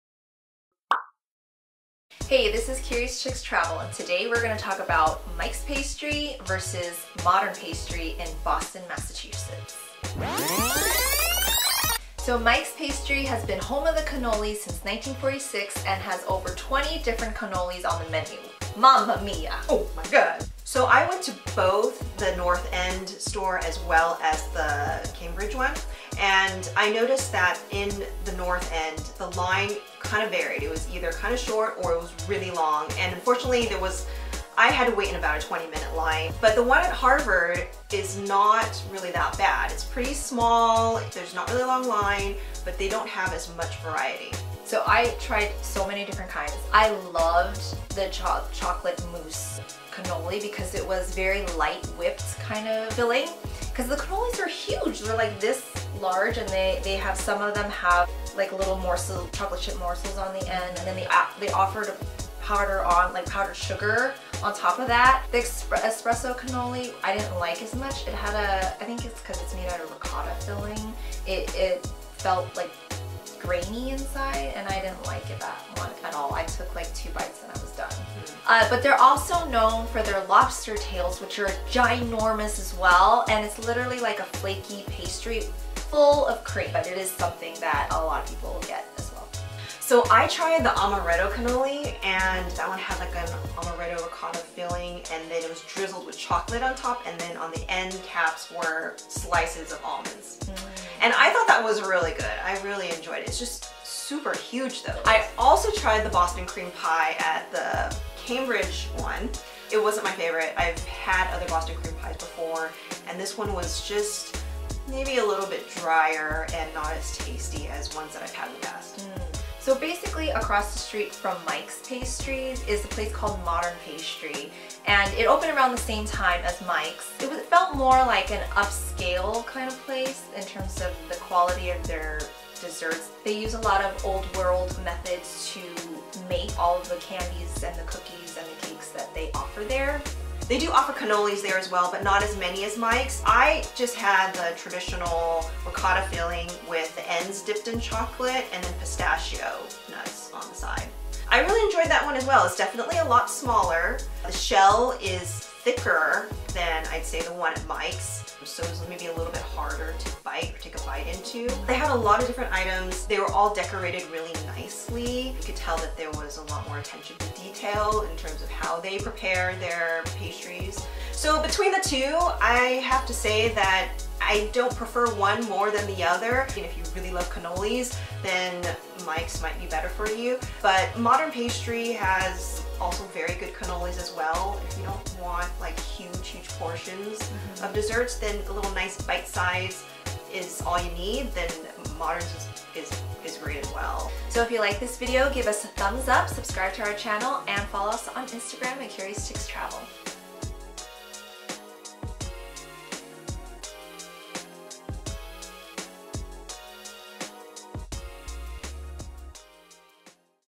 What's better? Mike's Pastry vs. Modern Pastry in Boston, MA
Boston is known for great food. This includes the cannolis at the famous Italian bakeries in the North End...Mike's Pastry and Modern Pastry. They reside across the street from each other on Hanover St. and both serve cannolis! But which one is better? Sienna and Carmen taste test both places tell you what they think. Before you head over to the North End which is practically a Little Italy, check out this video. ~Curious Chics Travel